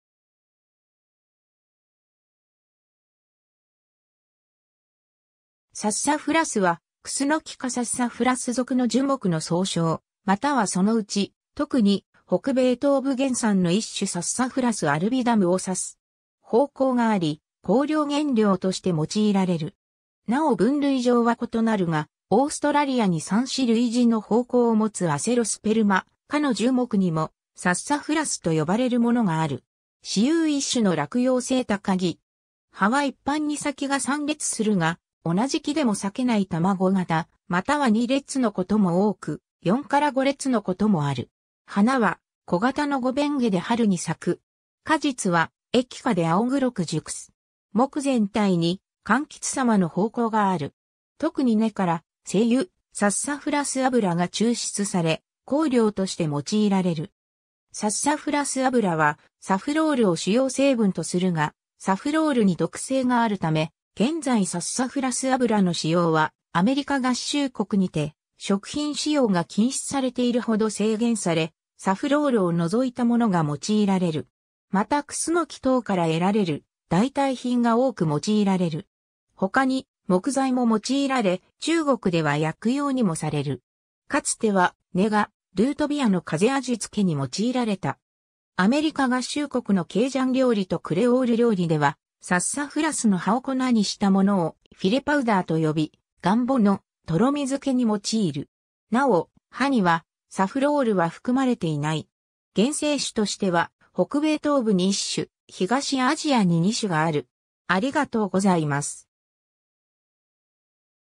サッサフラス
サッサフラス（Sassafras）はクスノキ科サッサフラス属の樹木の総称、またはそのうち特に北米東部原産の1種 Sassafras albidum を指す。
芳香があり、香料原料として用いられる。
 なお分類上は異なるが、オーストラリアに産し類似の芳香を持つアセロスペルマ科の樹木にもサッサフラスと呼ばれるものがある。
 雌雄異株の落葉性高木。
葉は一般に先が3裂するが、同じ木でも裂けない卵形または2裂のことも多く、4-5裂のこともある。
花は小型の5弁花で春に咲く。
果実は液果で青黒く熟す。
 木全体に柑橘様の芳香がある。
特に根から精油「サッサフラス油」が抽出され、香料として用いられる。
サッサフラス油はサフロールを主要成分とするが、サフロールに毒性があるため、現在サッサフラス油の使用はアメリカ合衆国にて食品使用が禁止されているほど制限され、サフロールを除いたものが用いられる。
またクスノキ等から得られる代替品が多く用いられる。
他に木材も用いられ、中国では薬用にもされる。
かつては根がルートビアの風味付けに用いられた。
 アメリカ合衆国のケイジャン料理とクレオール料理では、サッサフラス（S. albidum）の葉を粉にしたものをフィレパウダー（filé powder）と呼び、ガンボのとろみ付けに用いる。
なお、葉にはサフロールは含まれていない。
 現生種としては北米東部に1種、東アジアに2種がある。